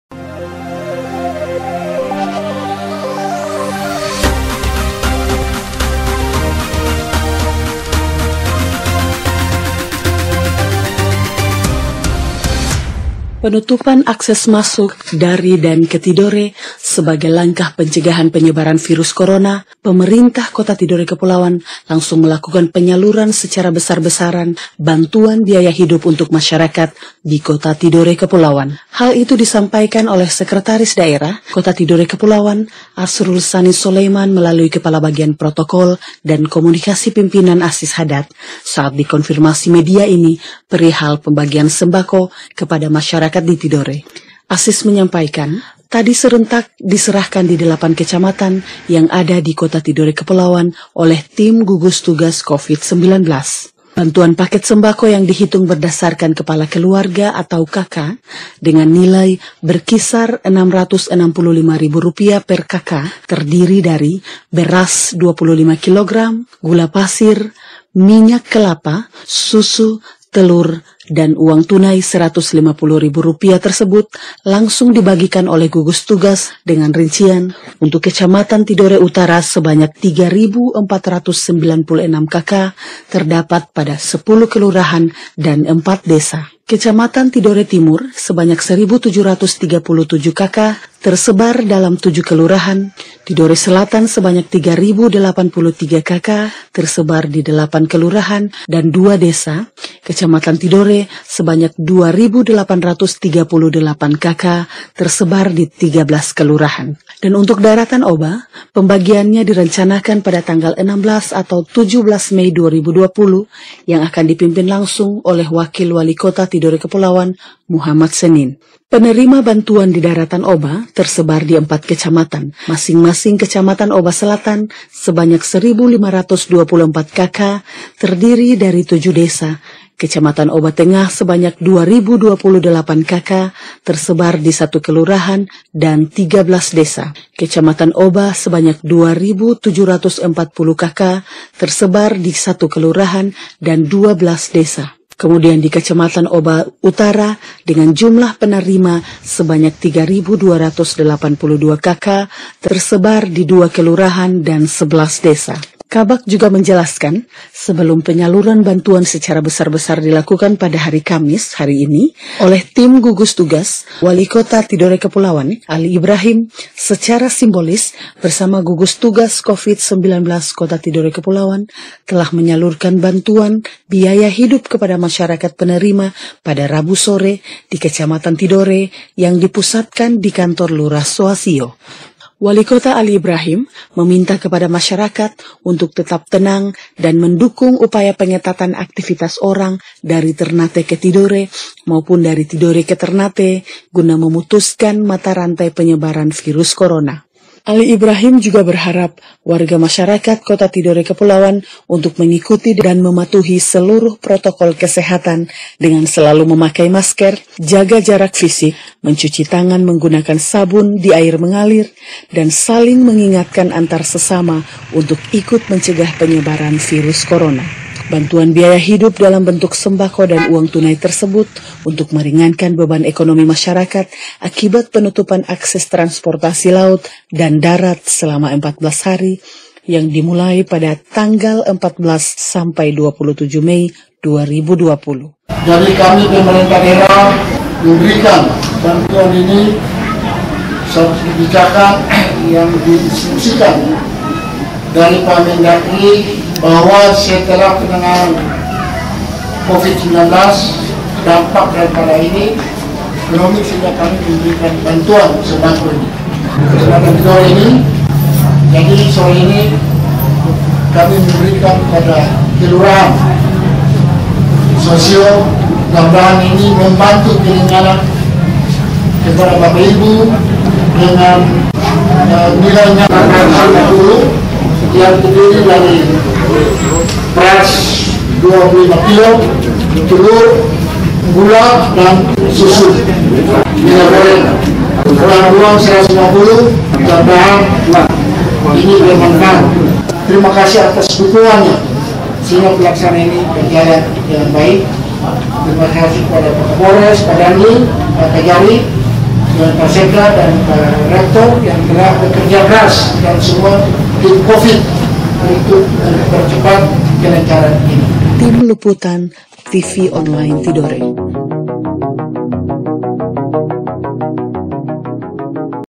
PEMKOT SALURKAN BANTUAN BIAYA HIDUP MASYARAKAT DI TIDORE
Bantuan paket sembako yang dihitung berdasarkan KK dengan nilai berkisar Rp 665 000 per KK diantaranya: Beras 25 Kg, gula pasir minyak Kelapa, susu, telur dan uang tunai Rp 150 000. Dibagikan oleh gugus tugas
Bantuan ini untuk meringankan beban ekonomi masyarakat akibat penutupan akses transportasi laut dan darat selama 14 hari yang dimulai pada tanggal 14 s/d 27 Mei 2020.
Walikota Tidore Kepulauan Ali Ibrahim, berharap warga masyarakat kota Tidore untuk tetap tenang dan mendukung upaya pengetatan aktivitas orang dari Ternate ke Tidore maupun dari Tidore ke Ternate  guna memutus mata rantai Virus Coroa.